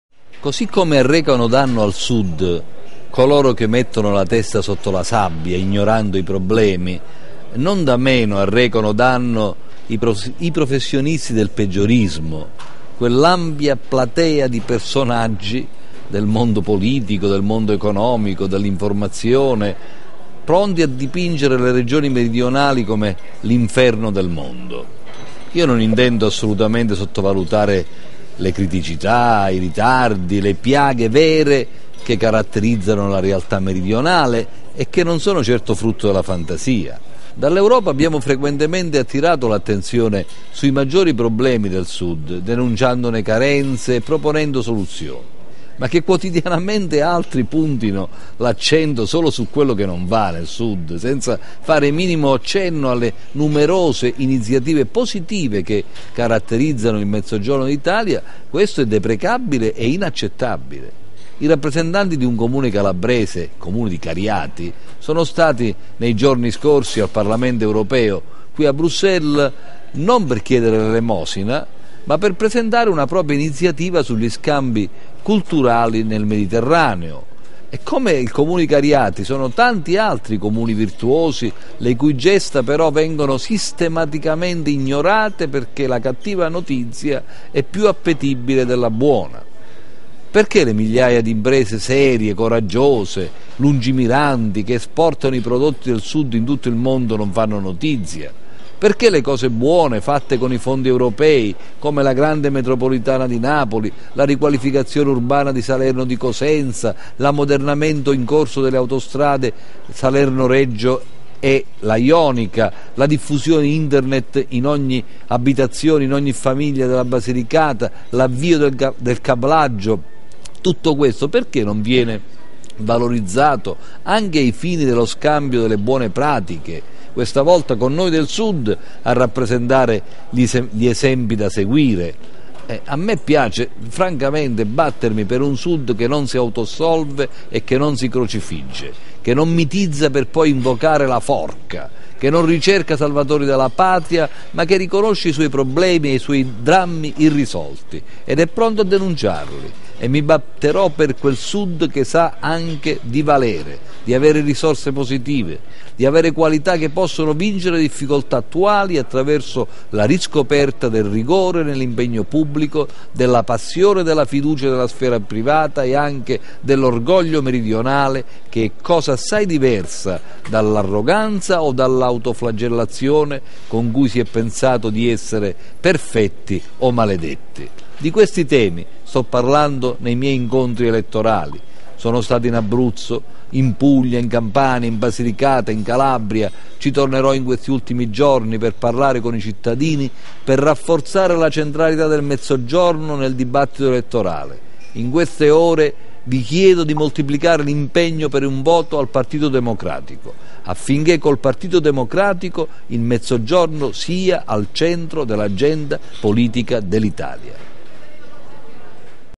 SUD: DENUNCIARE I MALI MA VALORIZZARE ANCHE LE POSITIVITÀ
Così come arrecano danno al Sud coloro che mettono la testa sotto la sabbia ignorando i problemi, non da meno arrecano danno i professionisti del peggiorismo, quell'ampia platea di personaggi del mondo politico, economico e dell'informazione, pronti a dipingere le regioni meridionali come l'inferno del mondo.